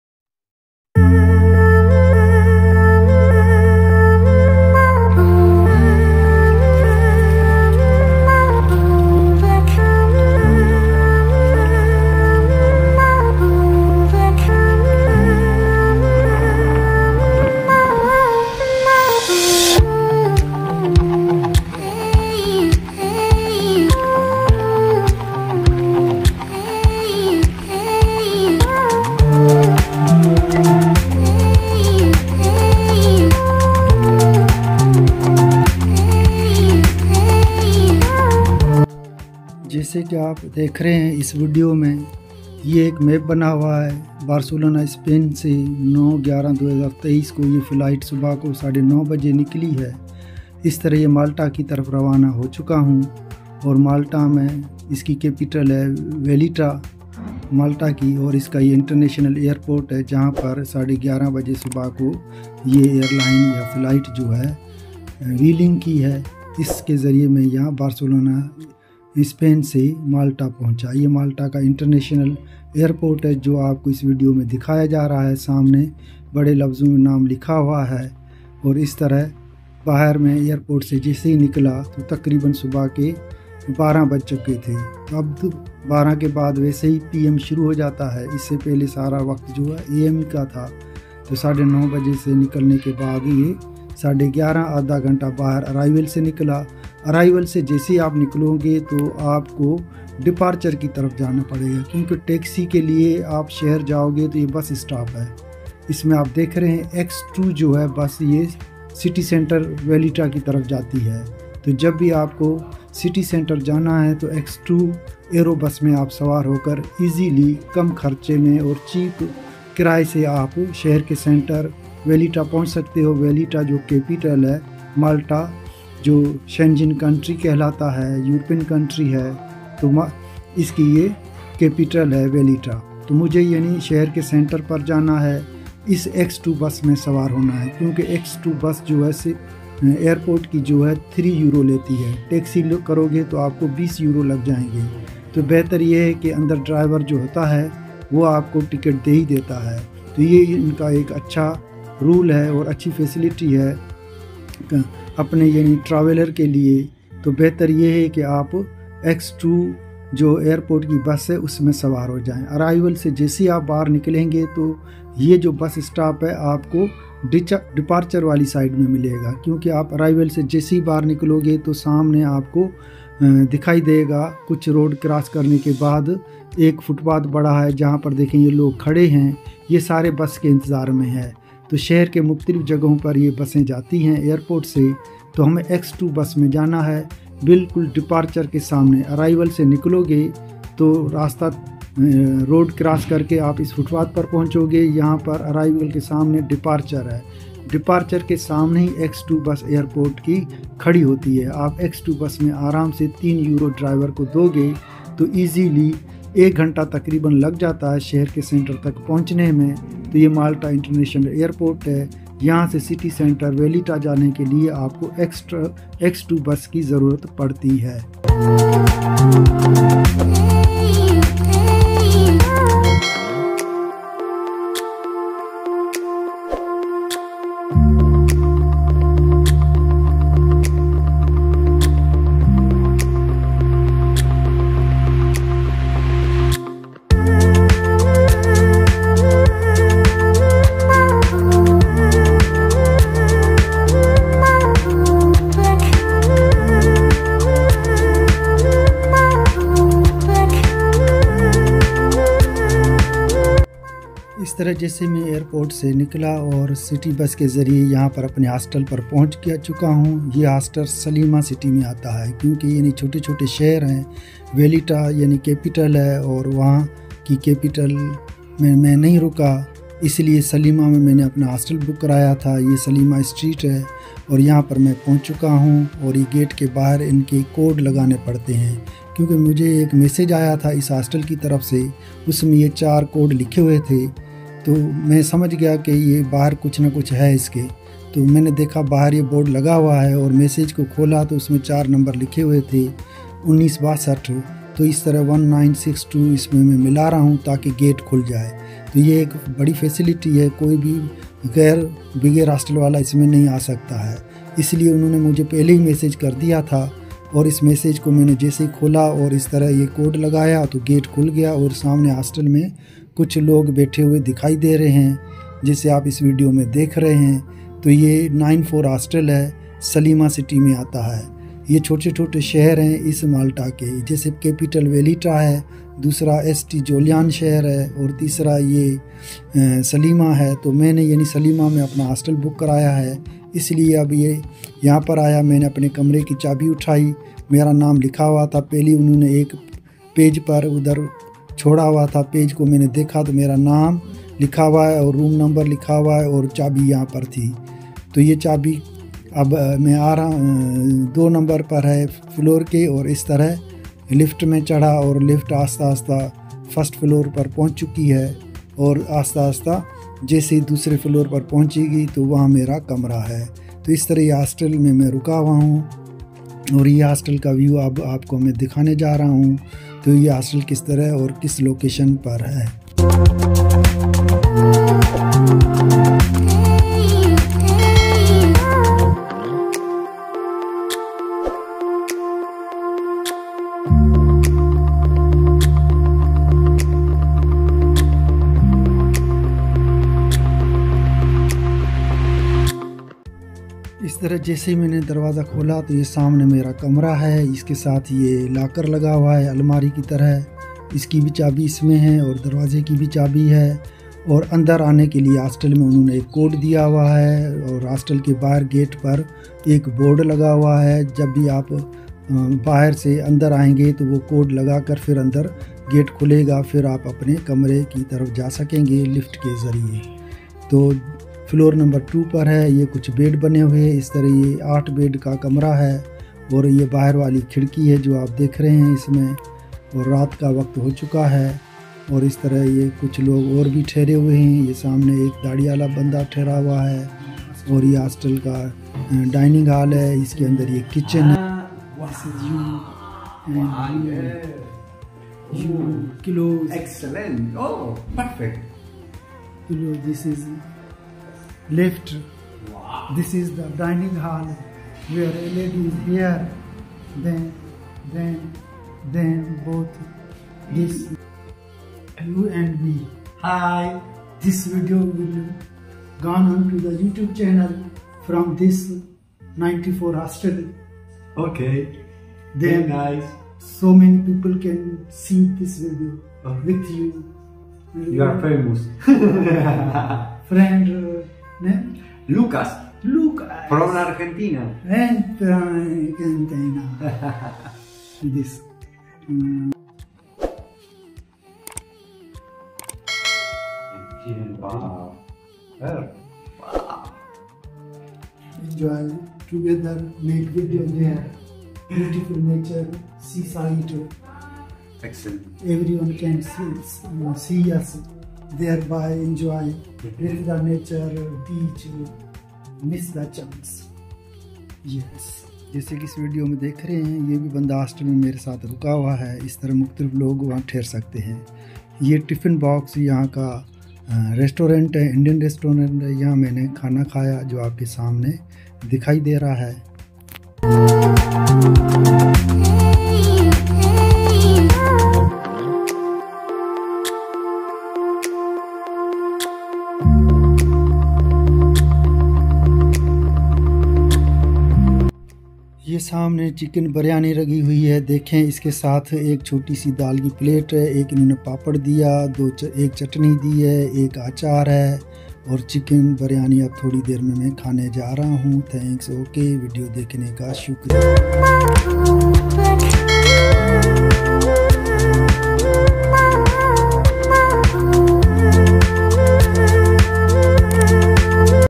Malta International airport, Valletta City,Malta @madanmalhi. How to stay and book hostel in Malta.
This video shows that how to walk around Malta International airport Valletta City, Malta and how to book and stay in hostel in Malta. Madanmalhitraveltheworld.